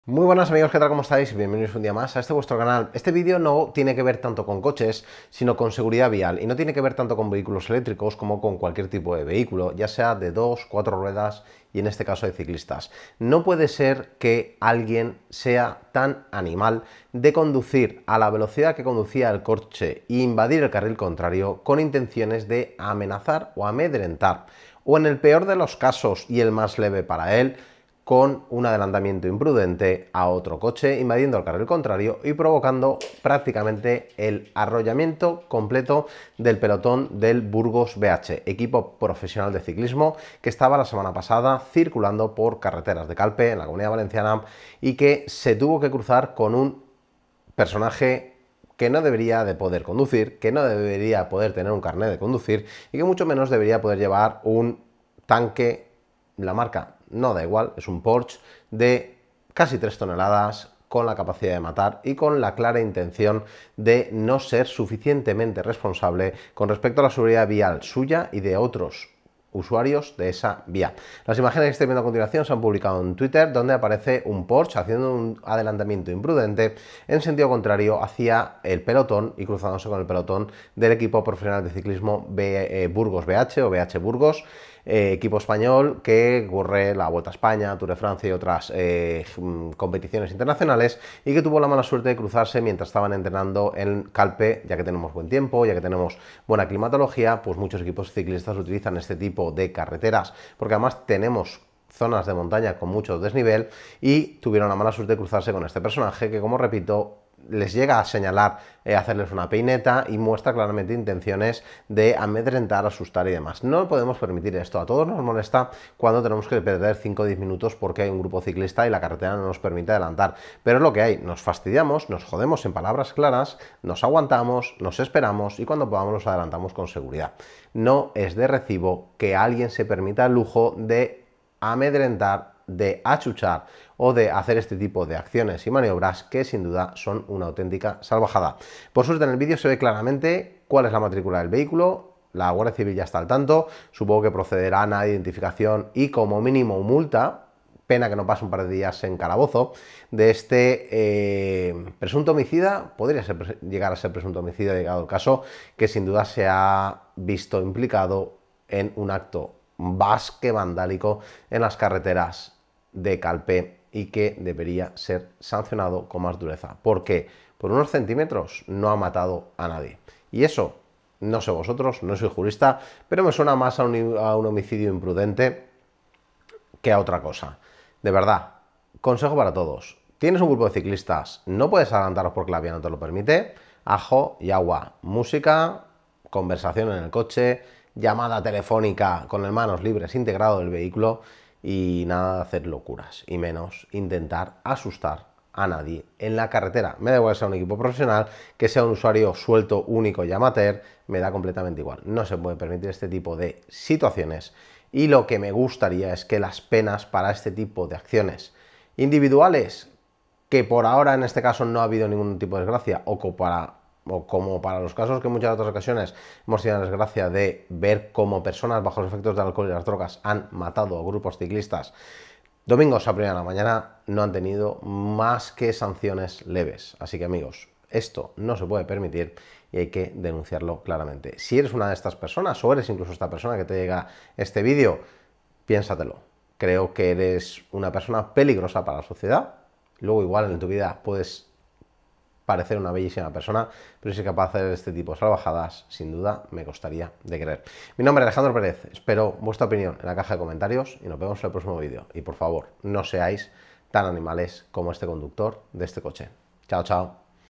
¿Asesino al Volante?: No cometas estos abusos!
Suceso acontecido en Calpe entre un conductor temerario y el grupo ciclista BH Burgos que casi fueron atropellados por un conductor.